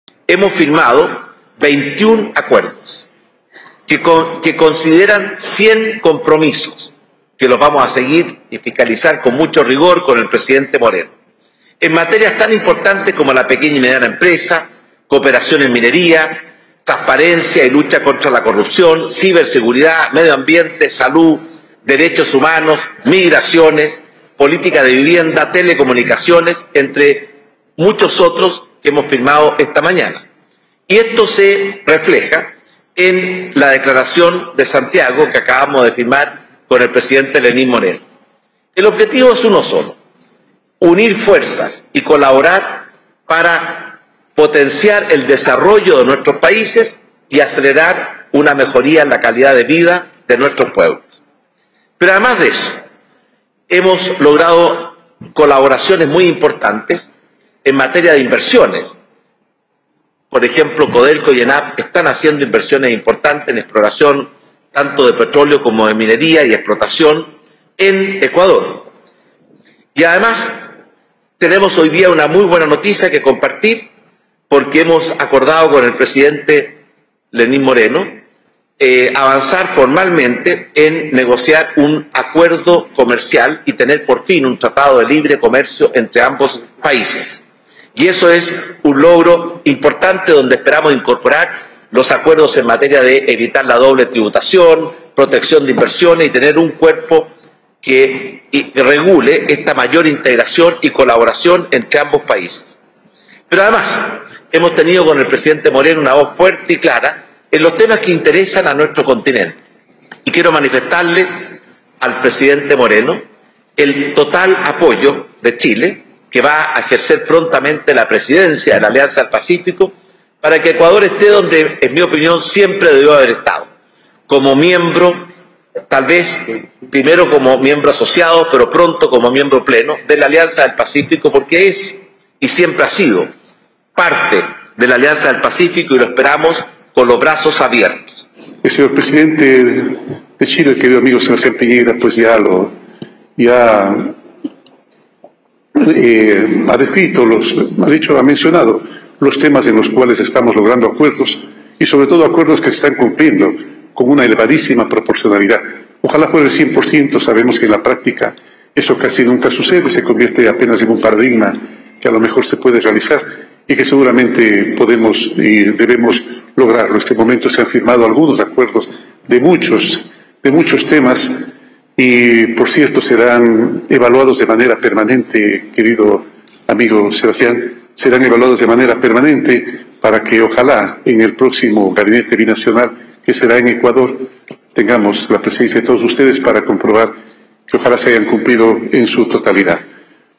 Ahora en DF: Chile acuerda avanzar en un tratado de libre comercio con Ecuador
Presidente Sebastián Piñera recibió en La Moneda al mandatario de Ecuador Lenin Moreno en el marco del V Encuentro Interministerial en el que se firmaron diversos acuerdos y se avanzó en coordinar un tratado de libre comercio entre ambos países.